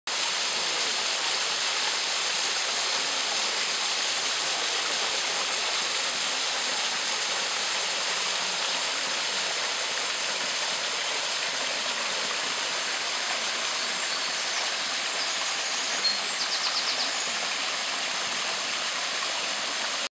Водоспад в Маліївцях
Водоспад в Маліївцях вражає своєю красою. Внизу кілька гротів роблять цей водоспад, особливим.